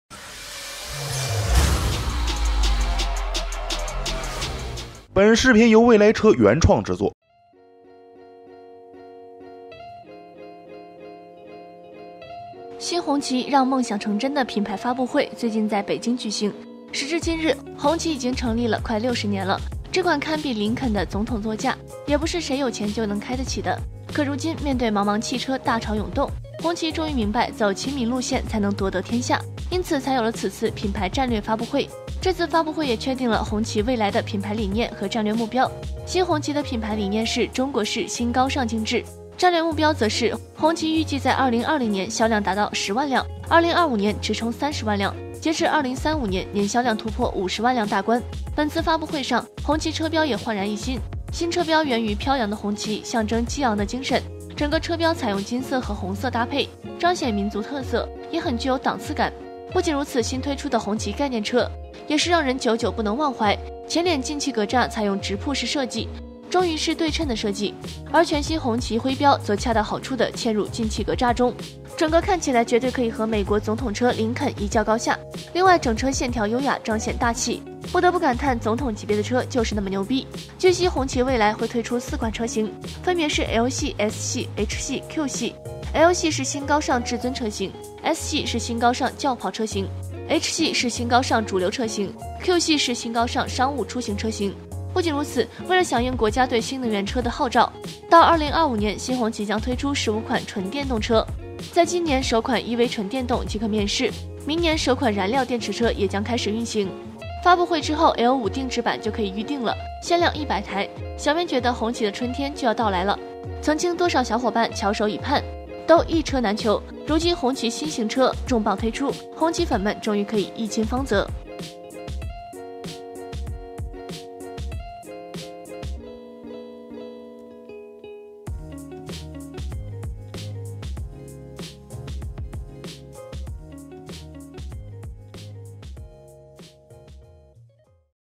紅旗2020年目標銷量10萬台，能做到嗎？
“新紅旗，讓夢想成真”的品牌發布會最近在北京舉行。時至今日，紅旗已經成立了快60年了，這款堪比林肯的總統座駕也不是誰有錢就開得起的。可如今，面對茫茫汽車大潮湧動，紅旗終于明白，走親民路線才能奪得天下。因此，才有了此次品牌戰略發布會。